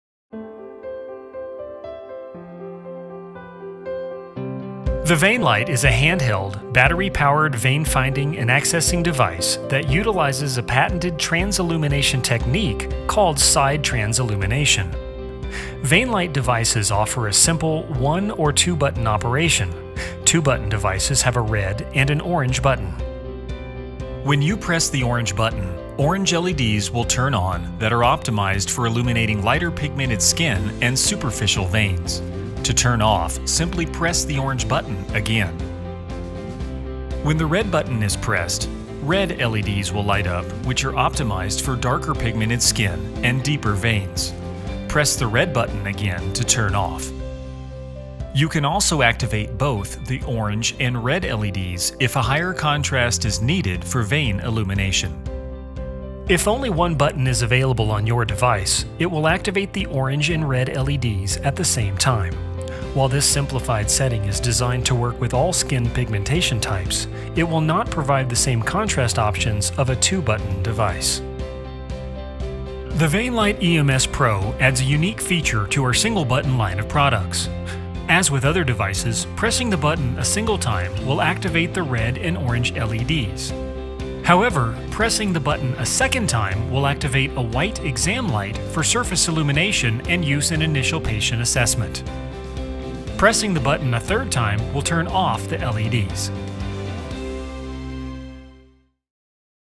Veinlite Device Functions
Short video on proper use of Veinlite devices. Covers both single-button and dual-button vein finders.